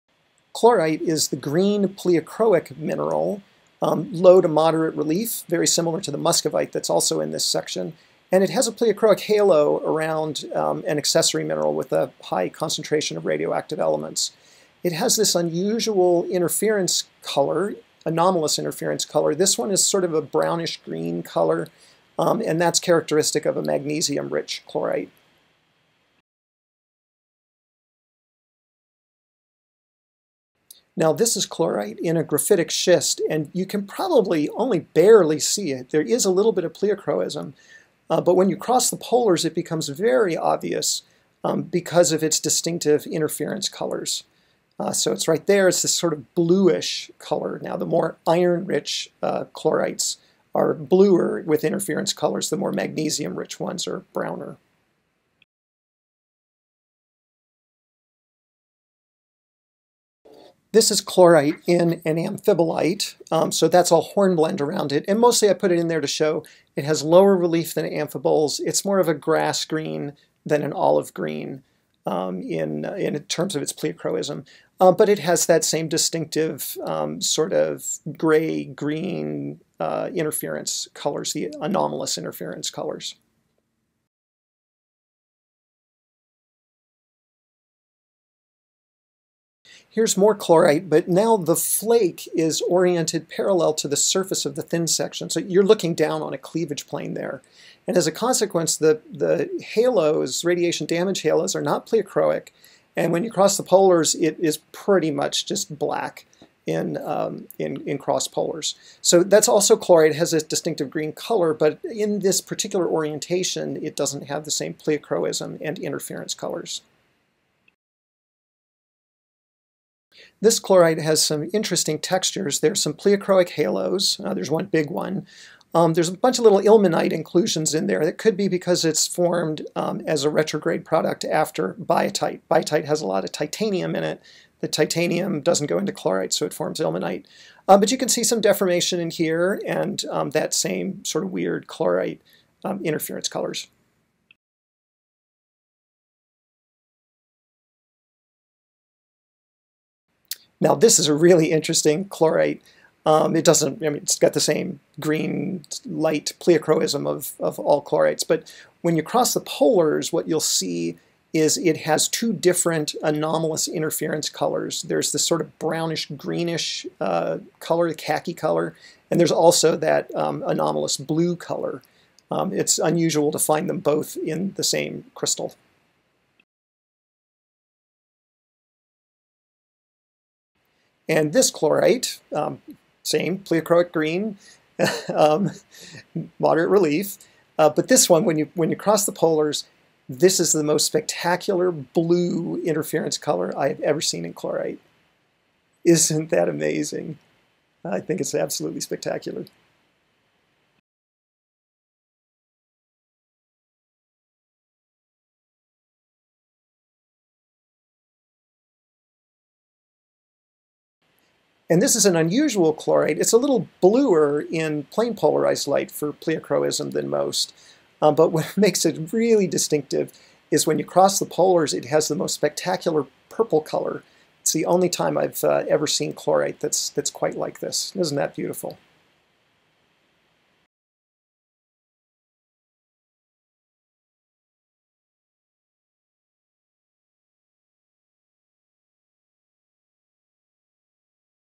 Chlorite tutorial Optical mineralogy
Several videos of chlorite in thin section, showing typical textures and optical characteristics. Supported by Boise State University and the US National Science Foundation.
Key characteristics: pleochroic in light green, low/moderate relief, pleochroic haloes, anomalous interference colors.
00:00 Typical single crystal with green pleochroism
00:30 Subtle pleochroism, anomalous blue interference colors
01:00 In an amphibolite (for comparison to hornblende)
01:30 Unusual orientation (looking down on a cleavage face)
02:00 Deformed crystal with ilmenite inclusions
02:30 Both anomalous brown/green and blue interference colors
03:00 Beautiful blue interference colors
03:31 Unusual purple interference colors